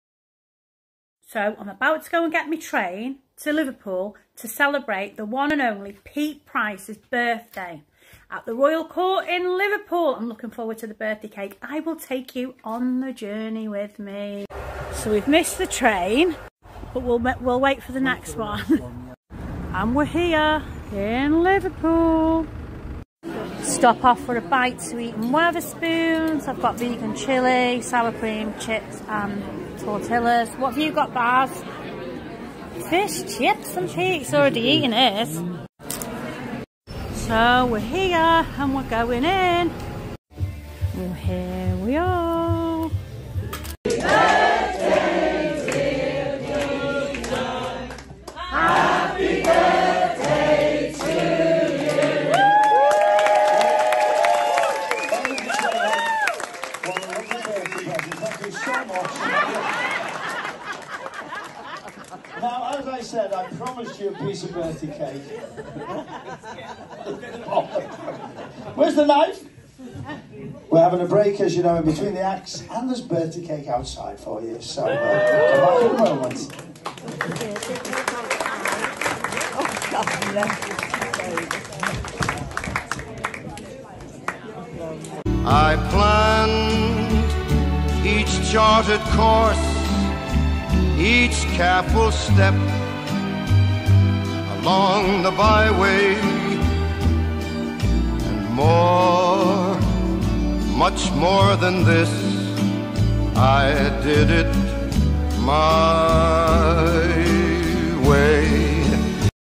Pete Price Holds Court and his Birthday celebrations 🍾
fantastic night of entertainment and celebrating legend Pete Price Birthday!